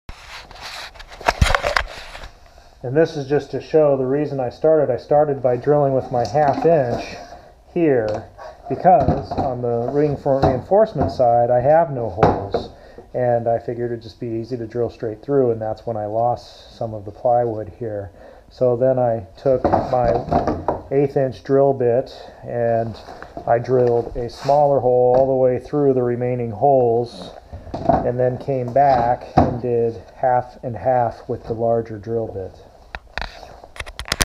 Building the P-51 Mustang Pedal Plane (Step 2, video 14 of 15)
For more details on how to build this P-51 Mustang pedal plane, CAUTION: Mistakes are explained in the video progression (watch all the videos in each playlist) or read the blog